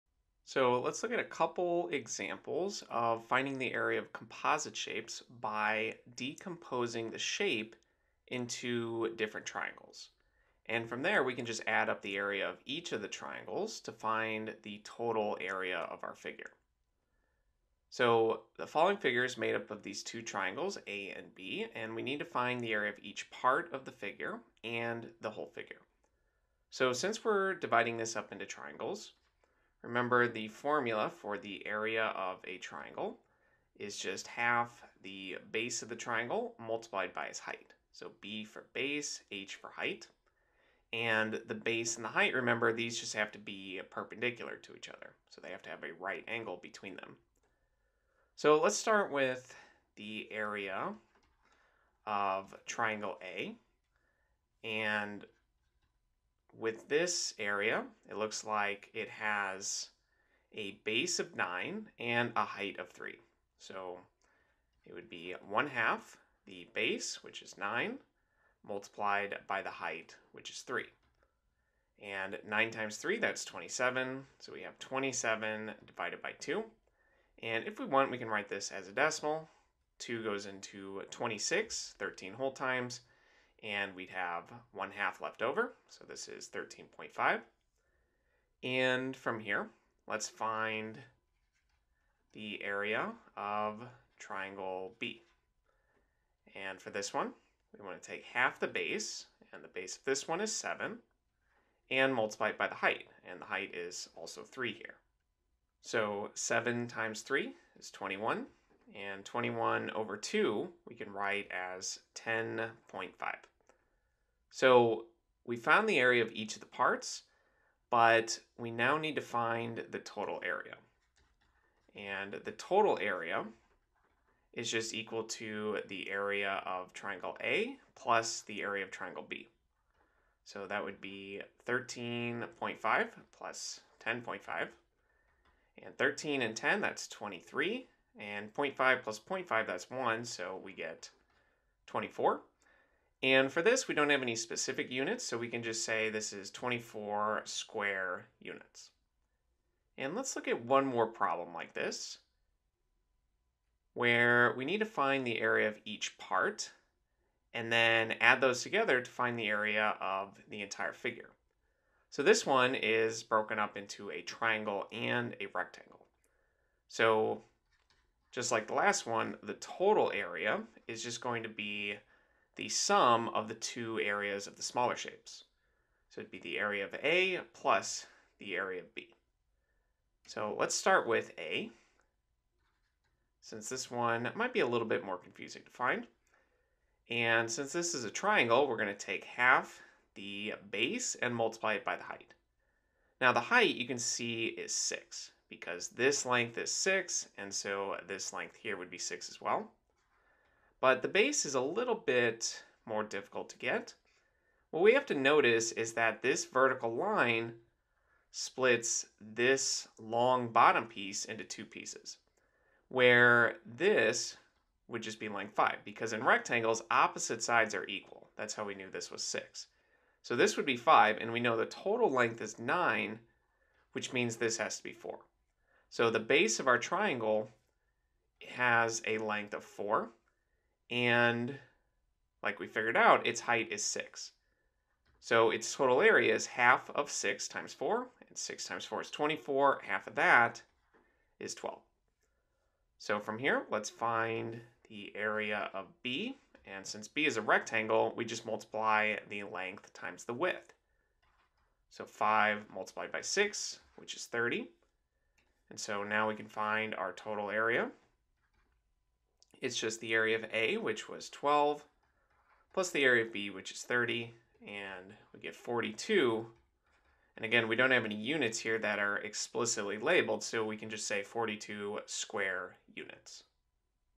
Area of Composite Shapes: Decompose Into Triangles
This video continues with composite shapes and how to find their area by looking at example problems from the Khan Academy exercise linked below on decomposing composite shapes into triangles.

A composite shape is a shape that is composed of several smaller, basic shapes. Often, these shapes are compositions of rectangles, triangles, squares, circles, etc.

To find the area of these composite shapes, break the shape up into smaller pieces where the area of each piece can be found. Once the area of the smaller division are found, add them together to find the total area.

Khan Academy exercises to practice:
1. Decompose Area with Triangles

2. Area of Composite Shapes

EulersAcademy.org